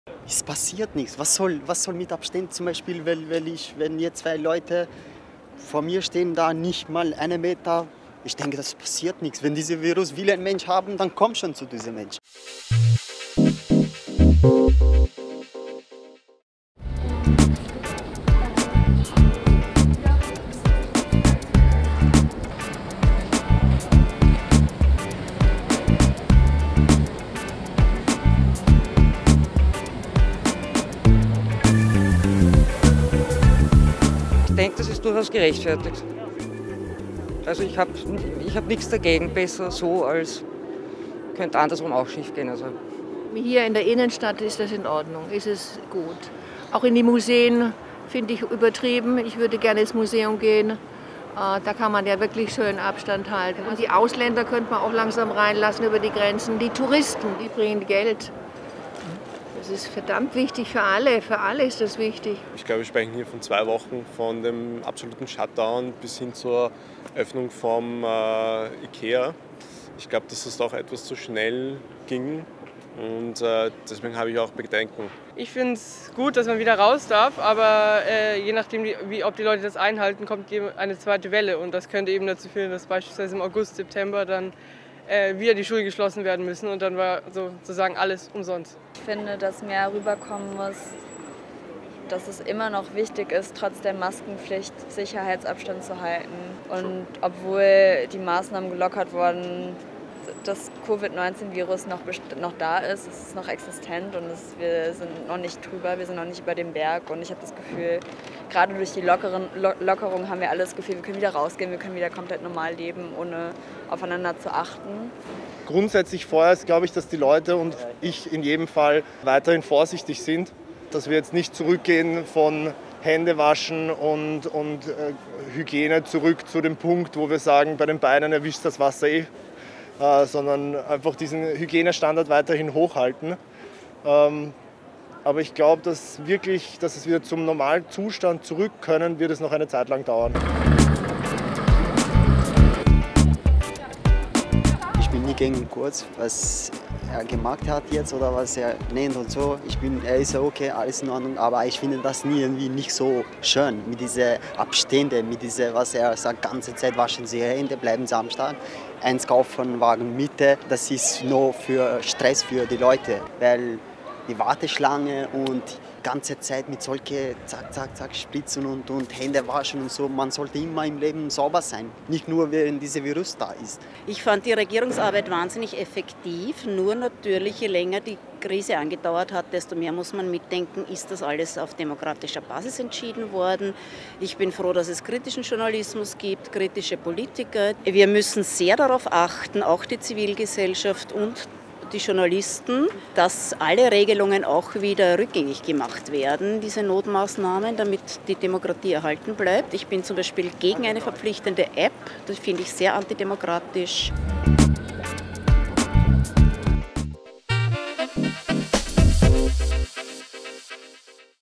Umfrage: Wie finden Sie die aktuellen Corona-Maßnahmen?
Mit Ende April sind die Ausgangsbeschränkungen in Österreich ausgelaufen. Seit 2. Mai dürfen alle Geschäfte sowie erste Dienstleister wie Friseure wieder öffnen. Ein Mindestabstand von einem Meter gegenüber haushaltsfremden Personen muss im öffentlichen Raum eingehalten werden. Was sagt man auf der Wiener Mariahilfer Straße zu diesen Maßnahmen?